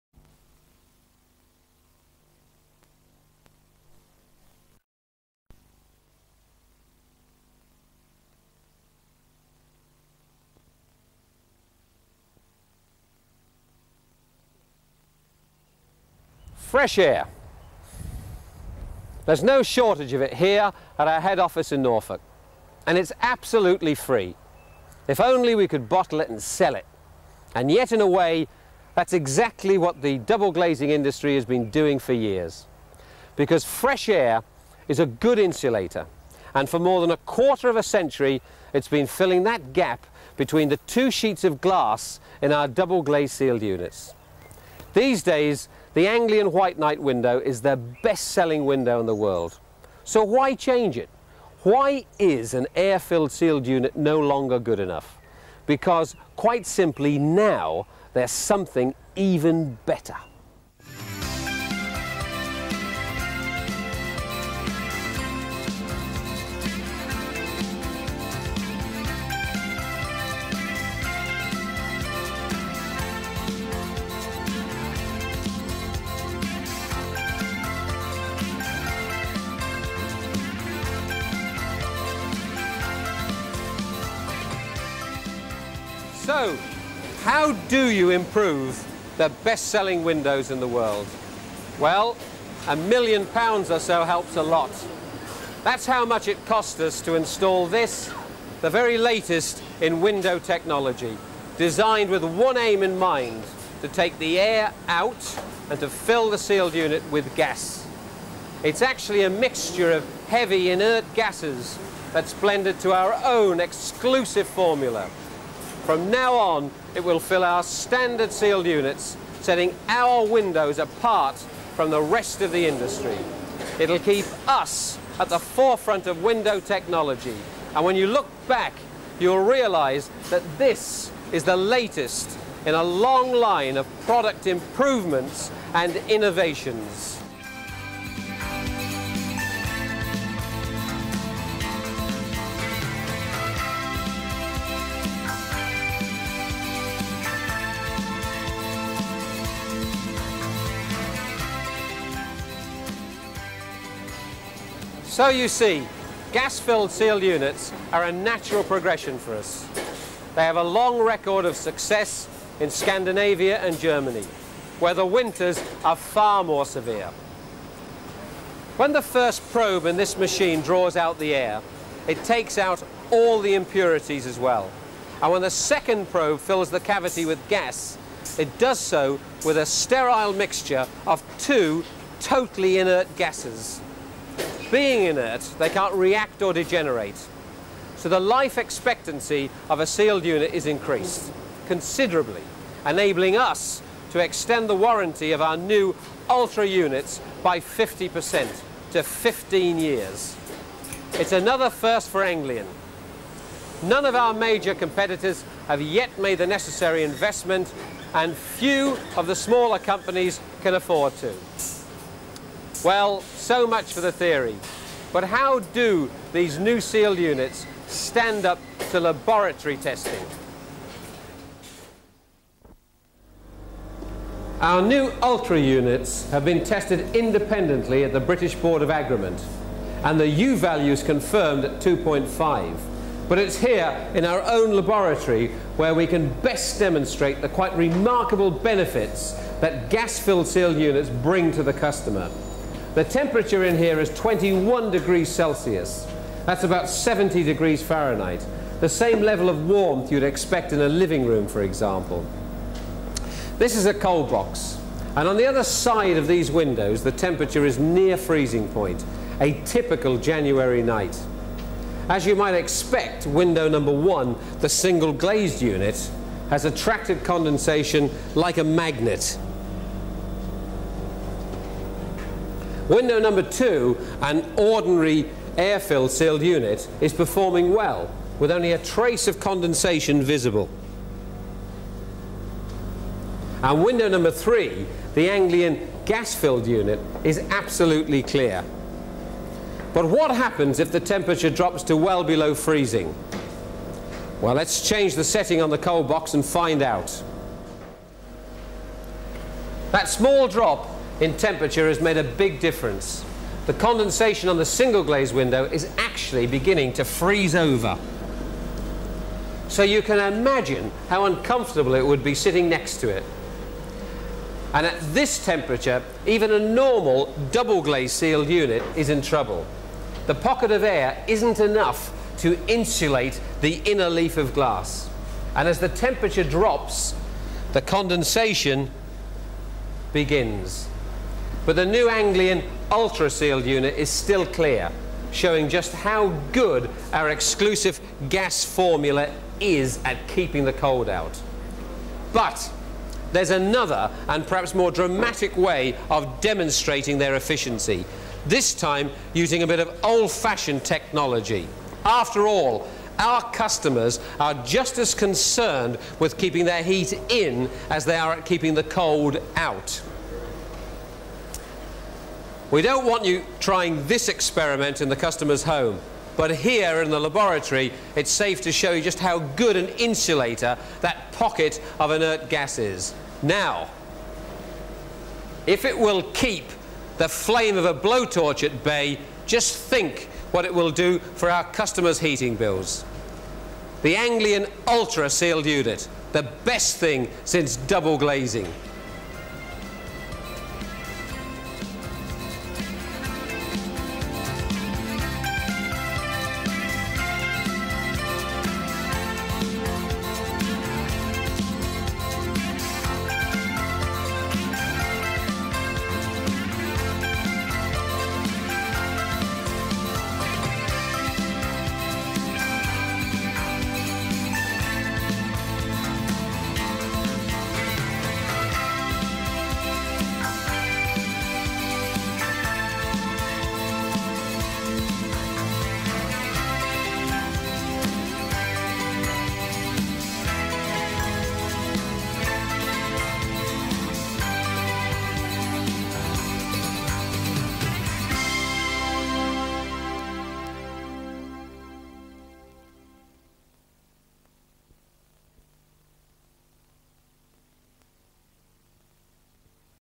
[Anglian Windows Training Video] The Best Thing Since Double Glazing
## I MADE A COMMENTARY!

Just over ten years ago, I worked for Anglian Home Improvements, and came across this training video, filmed when the company was converting from air-filled to argon-filled double glazed units. It's full of the typical training video stuff from the 1990s: cheap music, a fake laboratory, a marketing director who looks/sounds like Tony Bastable, and a stupid stunt that sadly doesn't go wrong.